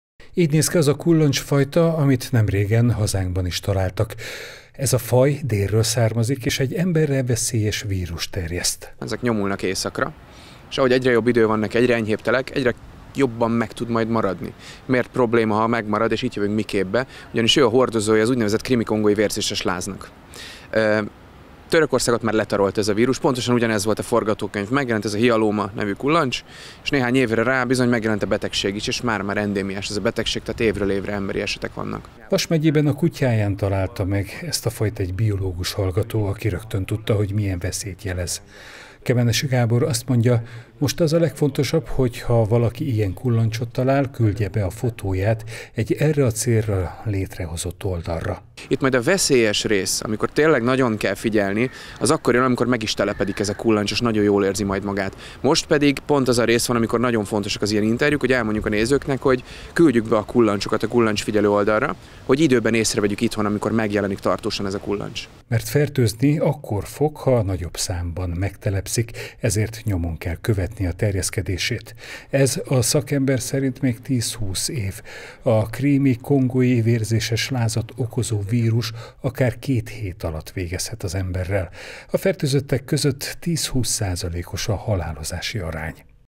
Veszélyes vírust terjeszthet - Új kullancsfajt találtak hazánkban
Új kullancsfajt találtak hazánkban, amelyik egy veszélyes betegség vírusát terjeszti. A Földközi-tenger partvidékének déli oldaláról, illetve Ázsia déli részeiről ismert ez a faj, amelyik egyre jobban északra húzódik, ahogy egyre jobban melegszik a klíma. A krími-kongói vérzéses láz kórokozóját hordja a kullancs, amelyik nem vár áldozatára, hanem keresi. Így szinte fut az áldozat után - mondja a szakember. A vérzéses láz egyébként igen veszélyes: a fertőzöttek 10-20%-a nem éli túl a betegséget.